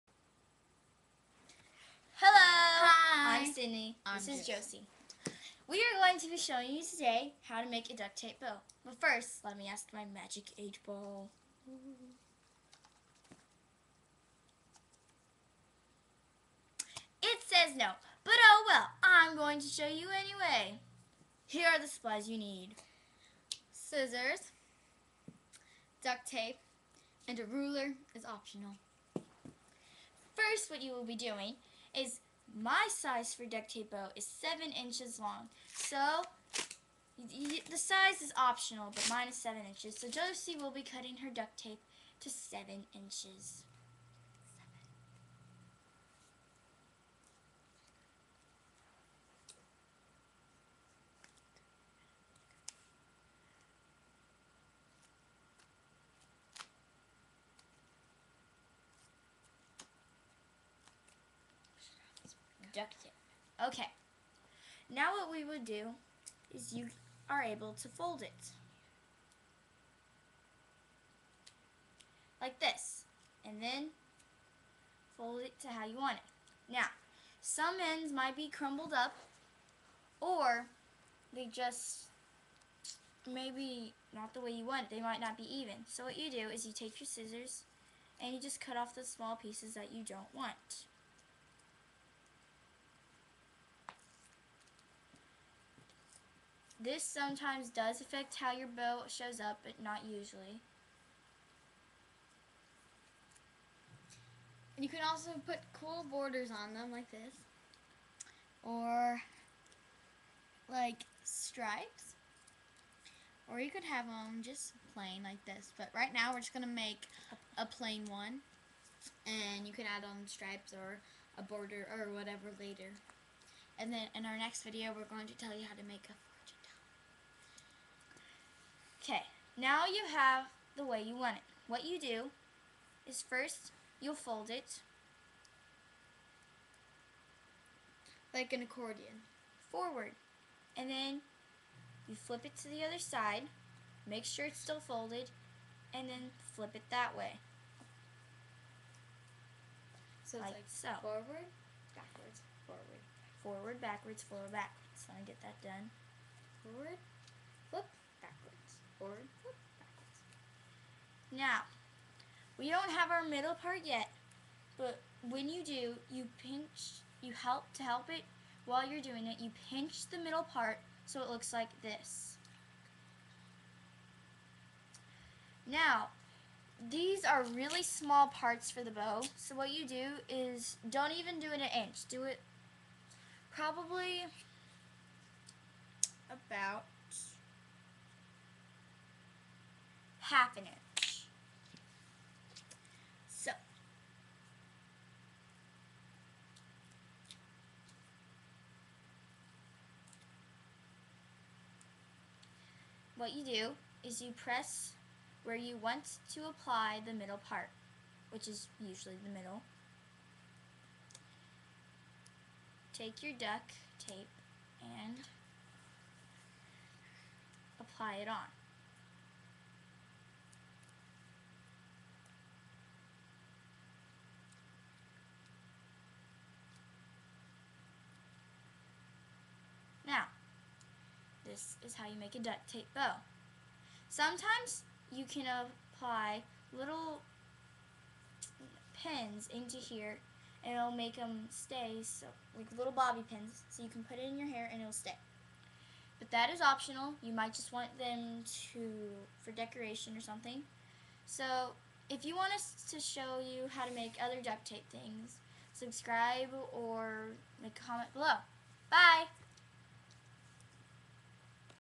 How to Make a Duct Tape Bow
When you are folding your duct tape fold it in half and cut the edges or pieces you don't want. Also thank you so much for over 100 views for out other video Hot Kool Aid